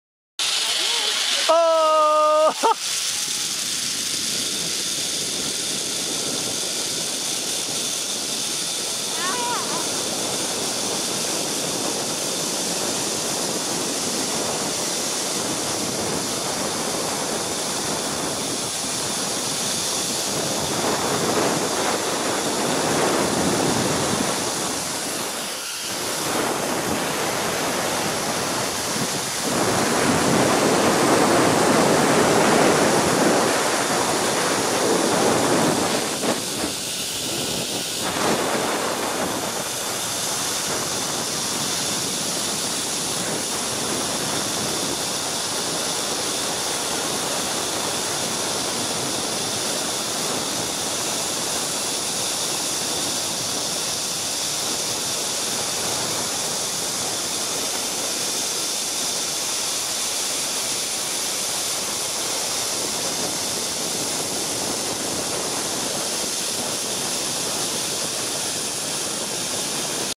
Superman Saskia
Ziplining the longest superman in Latam